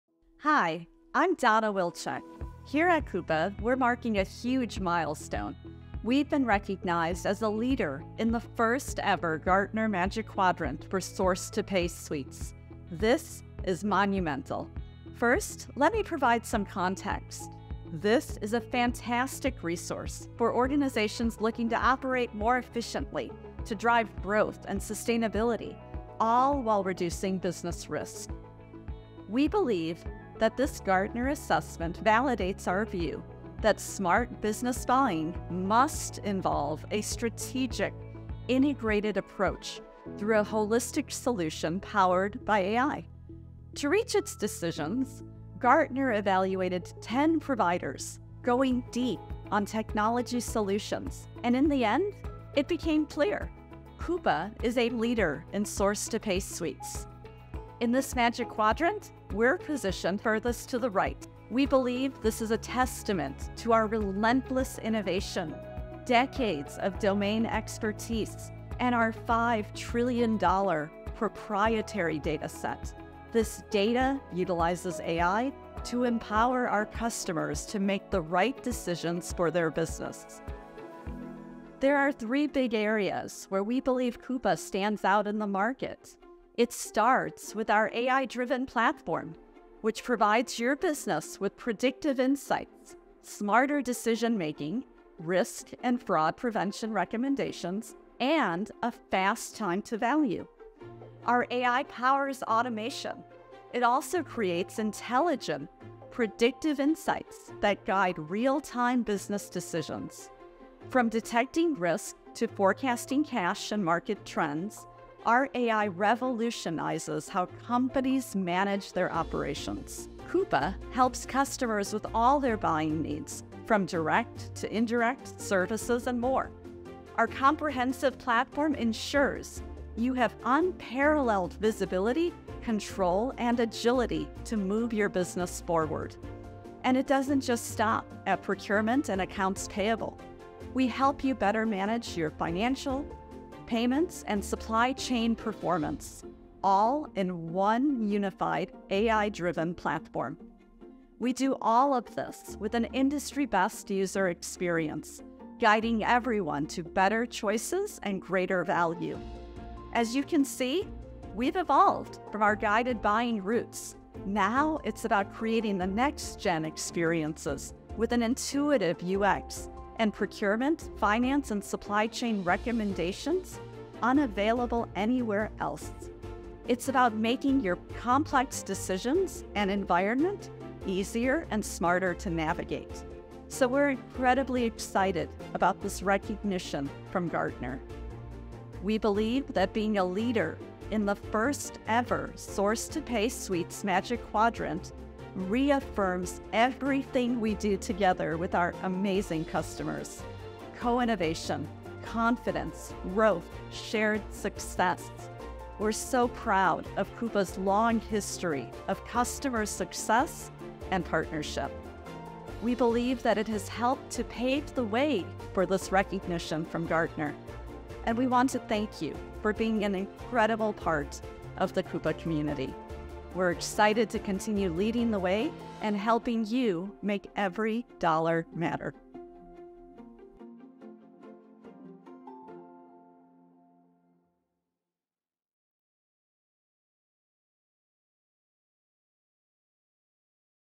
Coupa Named a Leader in 2024 Gartner® Magic Quadrant™ for Source-to-Pay Suites
2024  Complimentary Gartner® Magic Quadrant™ for Source-to-Pay Suites

Coupa is named a Leader for its completeness of vision and ability to execute in the first-ever source-to-pay (S2P) report.